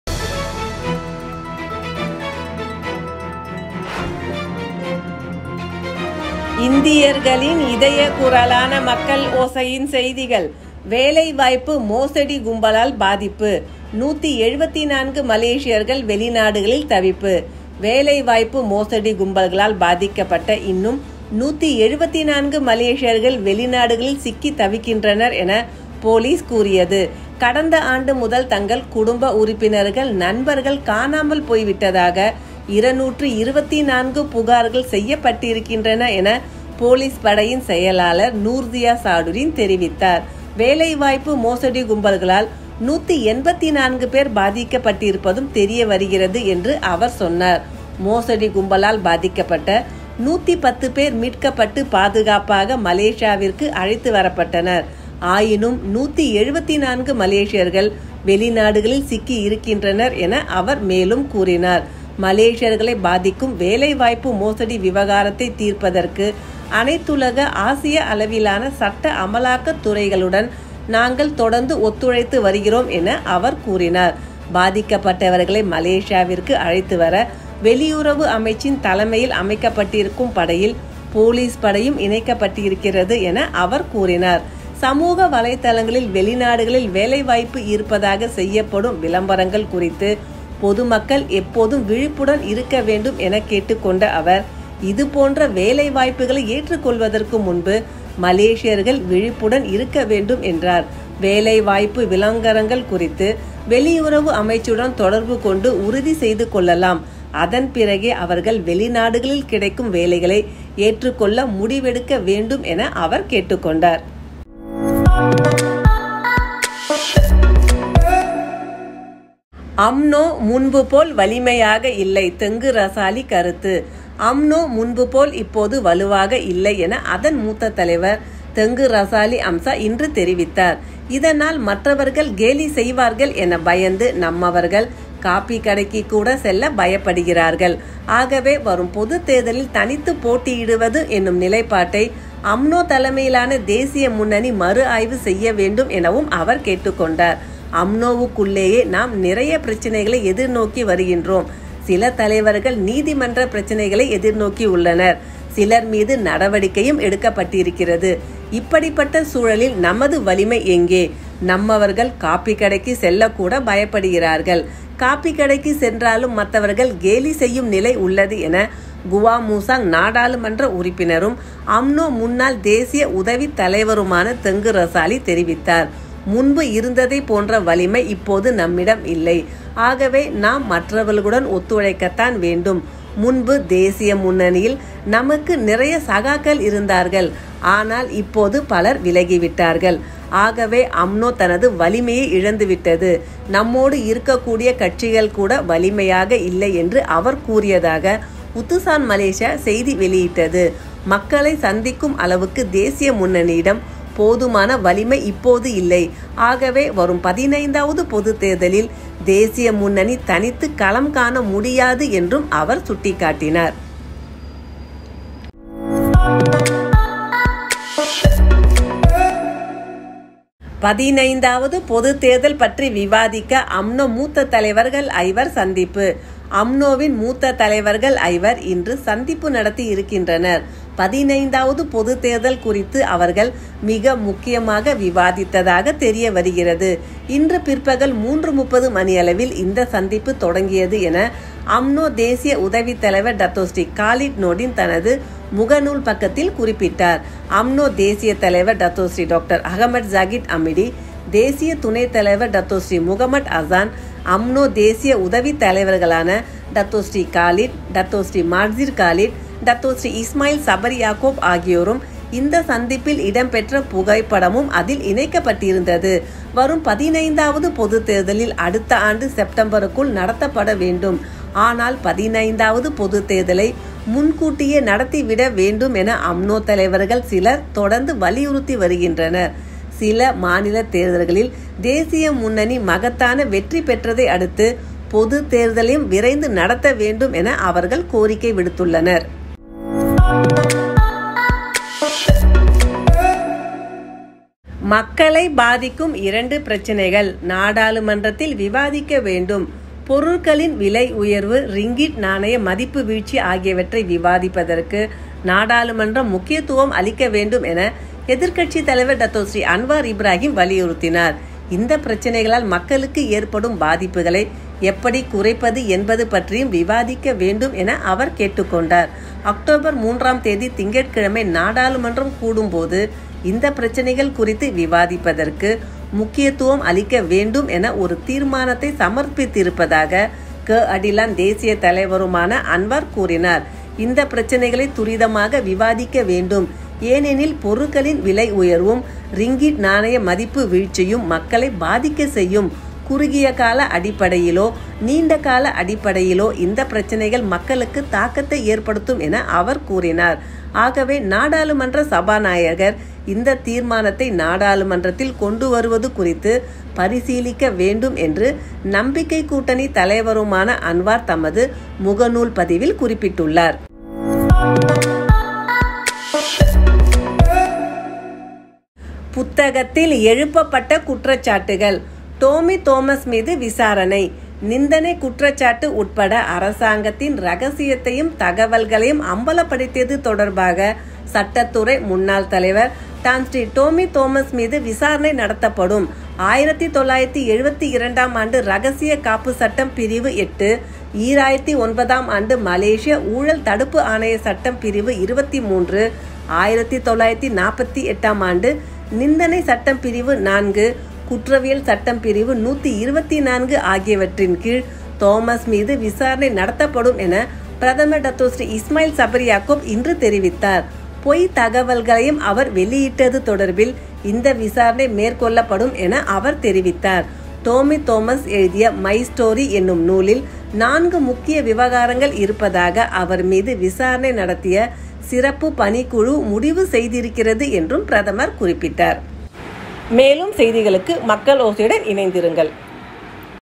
174 still held by job scam syndicates abroad, say cops
வேலை வாய்ப்பு  மோசடிக் கும்பாலால் பாதிப்பு